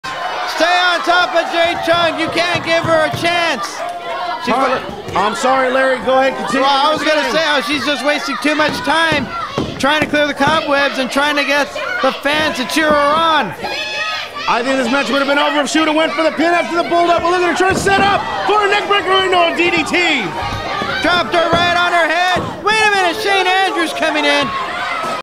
A Dazed Jade is Knocked Out and Pinned
After being in control for most of the match, Jade gets knocked for a loop.  Dazed and completely out of it, she's easy prey for her opponent to spike her on her head and finish her off!